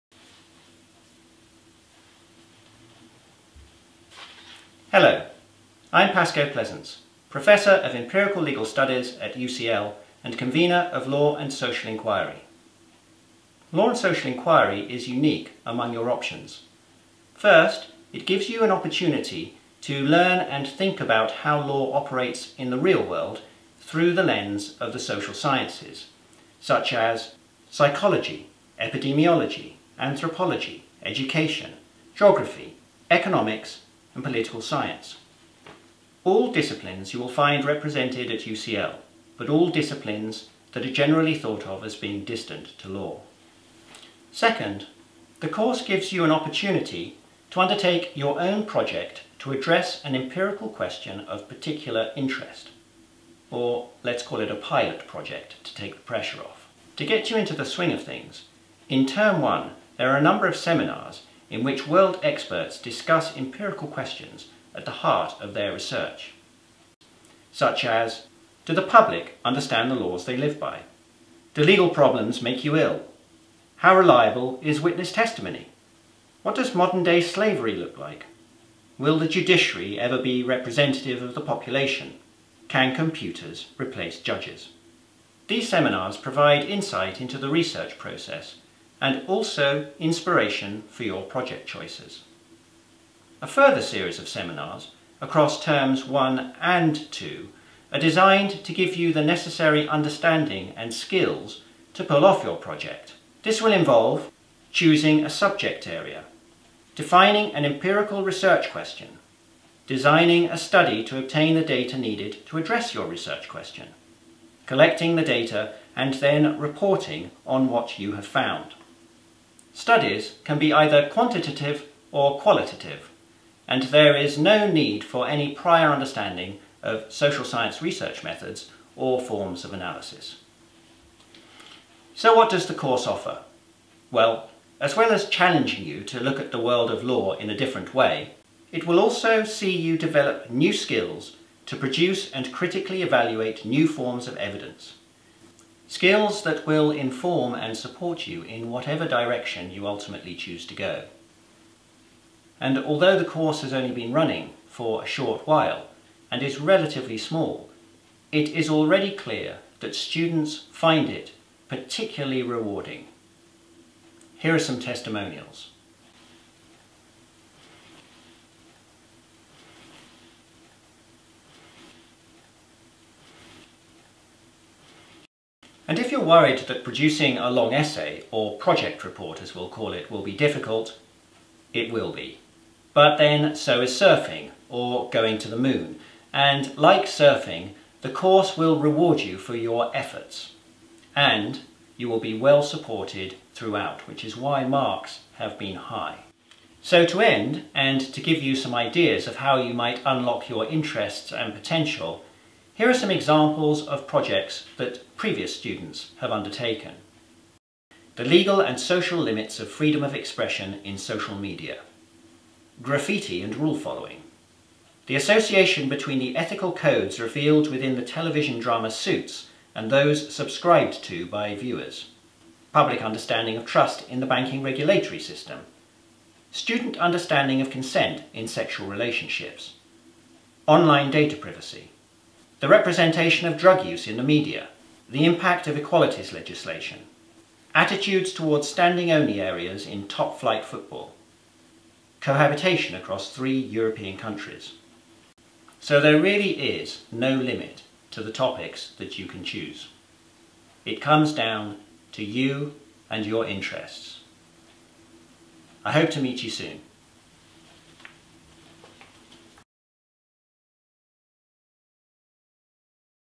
LAWS3047: Law and Social Inquiry // Professor Pascoe Pleasence
Law and Social Inquiry offers a unique opportunity to explore the intersection between the law and social sciences

In asking questions such as whether the public understand the laws they live by, how legal disputes are resolved in the real world, how reliable witness testimony is, what modern day slavery looks like, whether the judiciary will ever be representative of the population, and if juries are biased, is to ask empirical questions. Empirical questions are the subject matter of the social sciences, and answering such questions requires the application of social science research methods. These have evolved within various disciplines –  including anthropology, criminology, epidemiology, political science, psychology and sociology – and provide a diverse range of approaches to social inquiry in the field of law. Law and Social Inquiry gives you time and space to learn about this diverse range of approaches and further your legal understanding through the application of one or more of them.